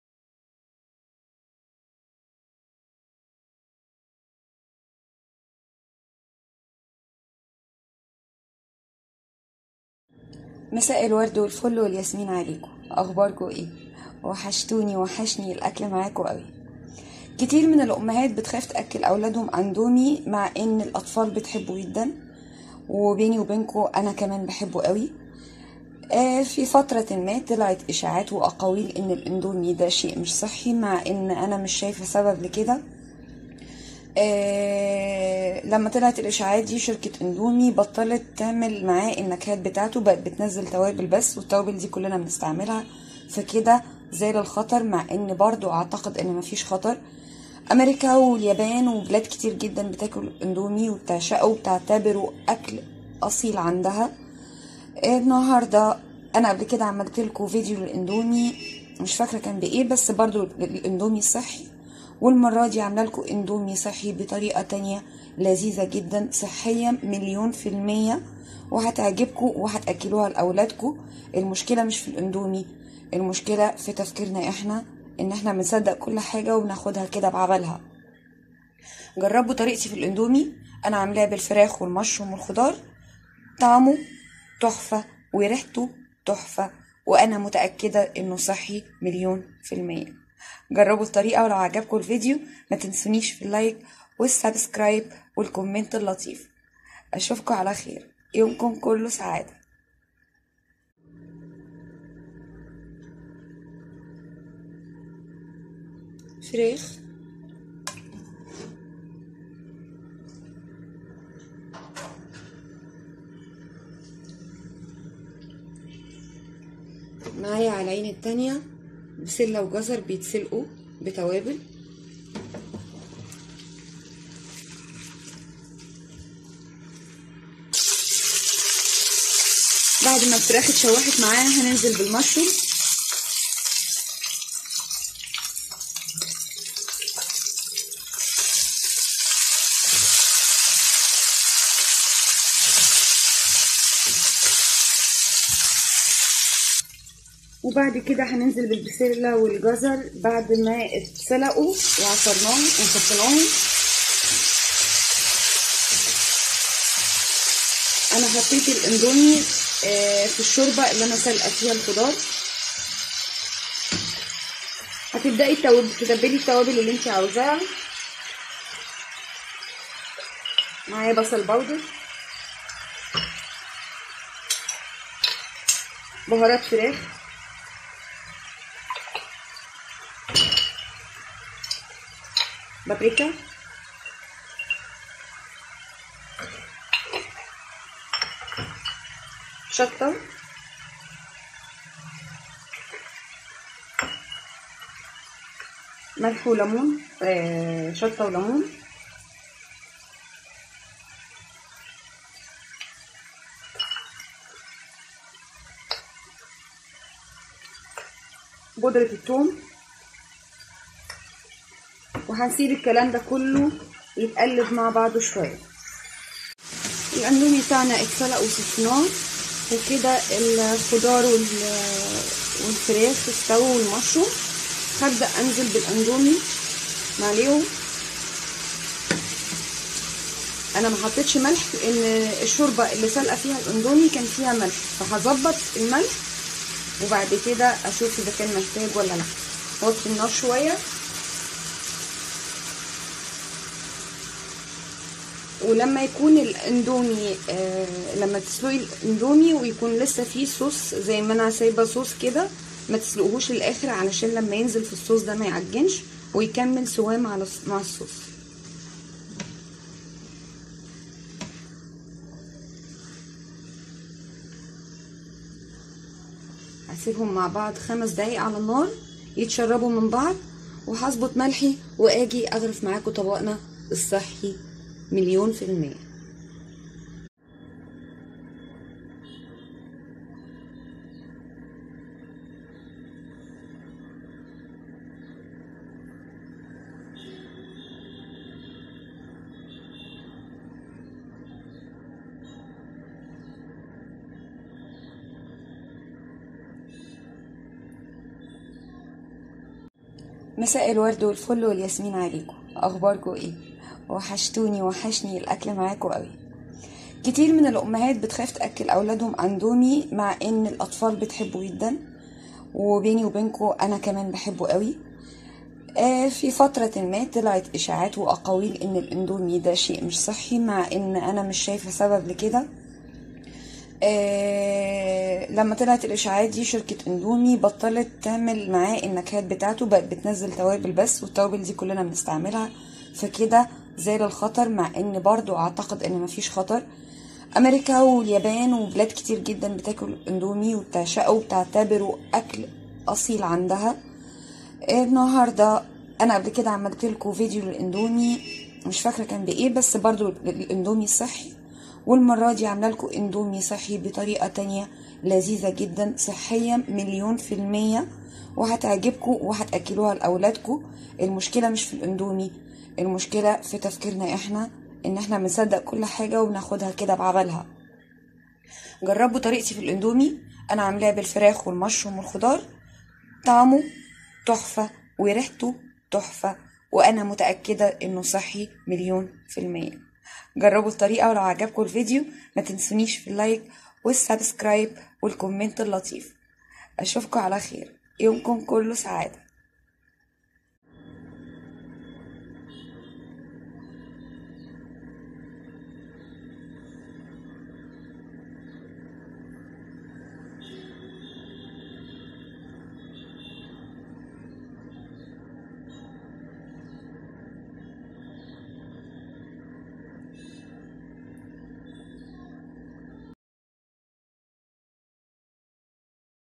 خايفه علي اولادك من الاندومي؟؟ازاي تعملي الاندومي او النودلز بطريقه صحيه و طعم خرااافي
خاص بالمرأة و حياتها و مساعدتها عالحياه
الاندومى الصحى,